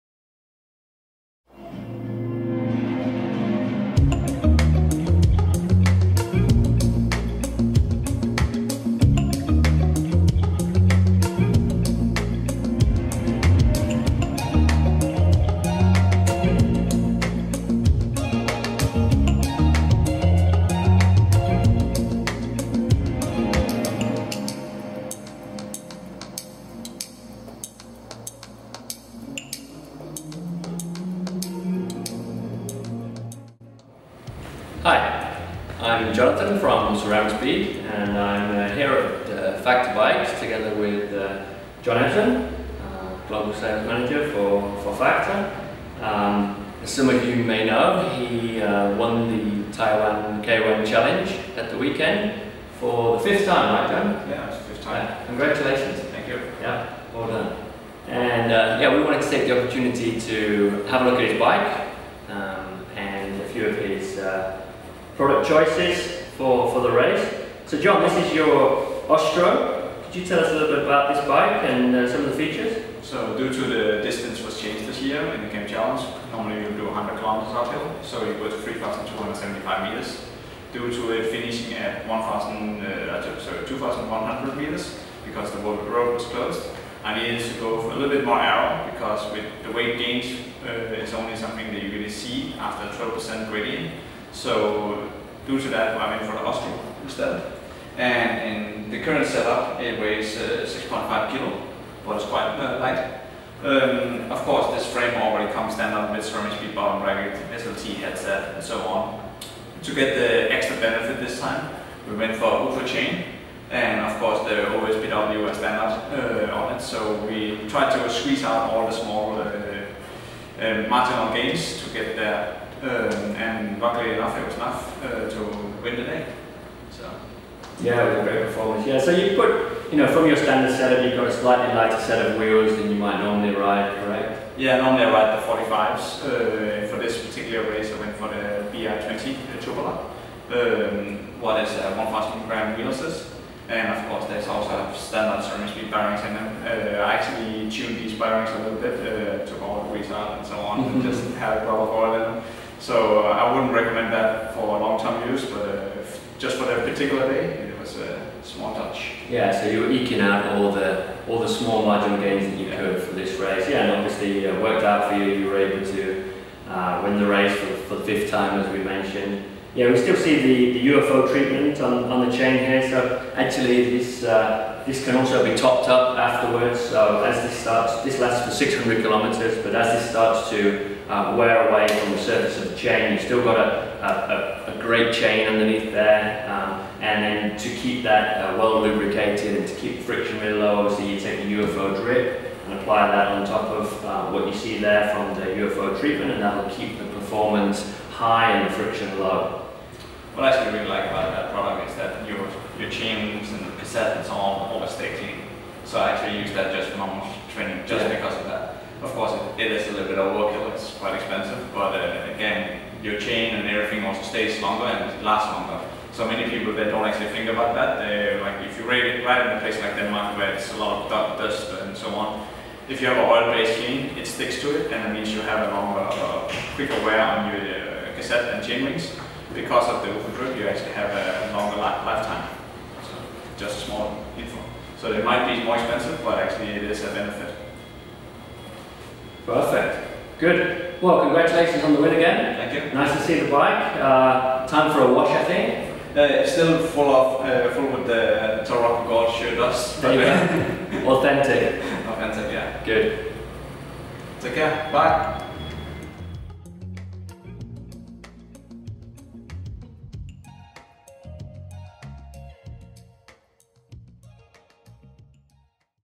Ceramicspeed - Factor John Ebsen 的台灣 KOM 冠軍單車
Factor 的全球銷售經理 John Ebsen 在 Ceramicspeed 的邀請之下介紹了這次幫助他獲勝的單車，來看看他如何選擇車上的每一個零件與擠出每一個轉動部件的最高性能與效益，只為讓他可以更靠近一點頒獎台，並且取得勝利。